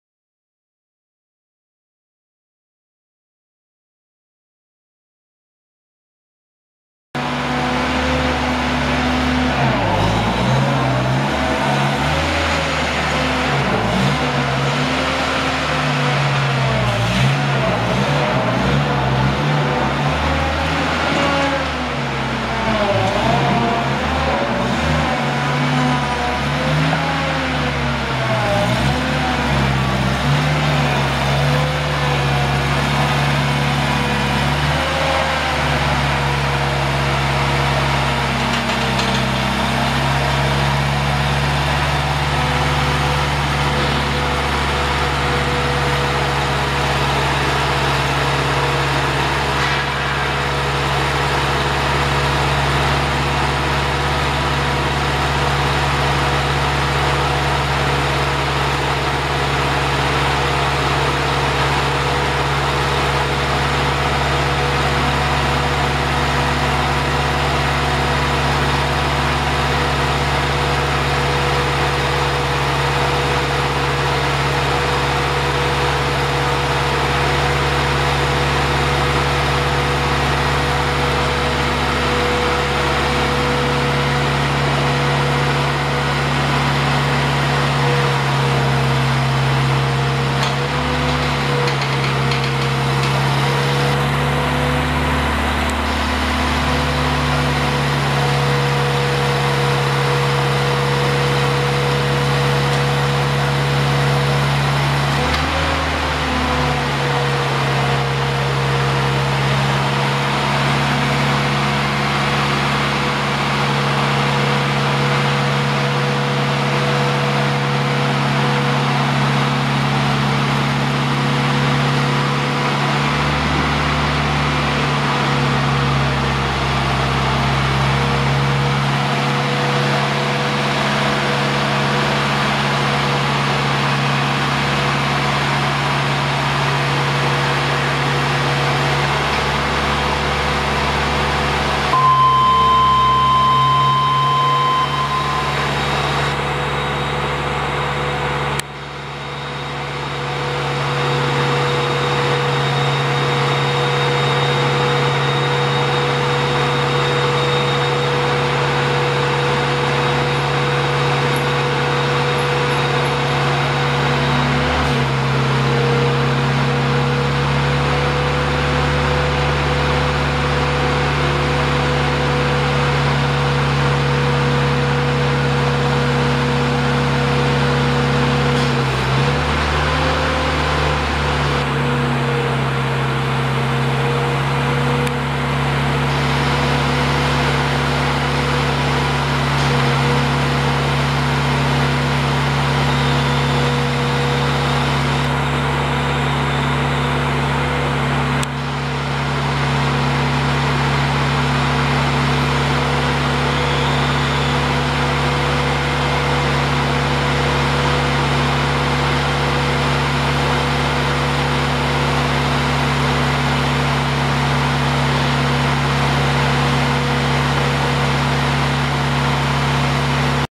1986 Genie Z45/22 articulating boom lift for sale | sold at auction February 19, 2014
1986 Genie Z45/22 articulating boom lift
2,905 hours on meter
Ford LRG-423 four cylinder dual fuel engine
LP and gasoline
Four wheel drive
500 lbs work load capacity
45&#039; maximum platform height
23&#039; maximum platform reach
70" x 30" platform
45"H guard rail
Ground and platform controls
Ground and platform 115V AC outlets
Outriggers
14-17.5 NHS tires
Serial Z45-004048

It was located in Park City, KS, and we sold it to the highest bidder for $20,900.